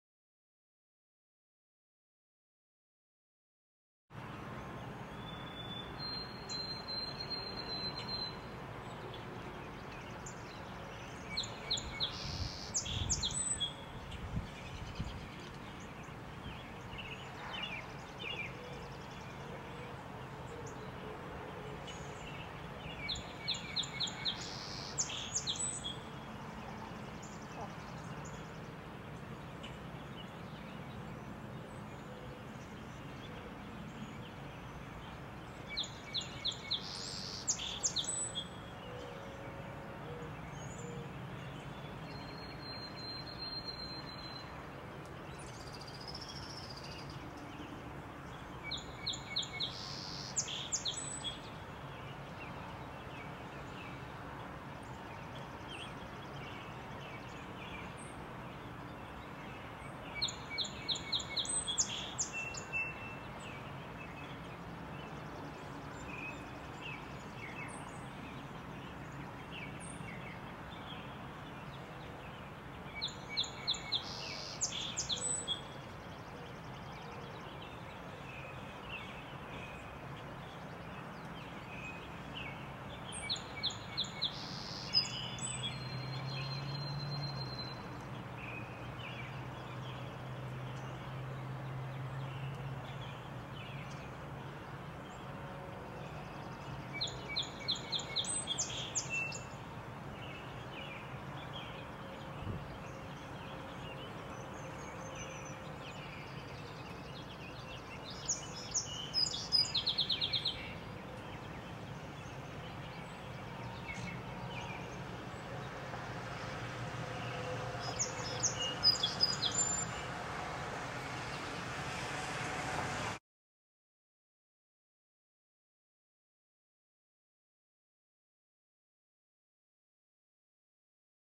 Song Sparrow
Just "discovered" this beautiful little songster!  Here's a little sample on a cloudy Minnesota morning in April.  Enjoy.
Note the White-Throated Sparrow in the background.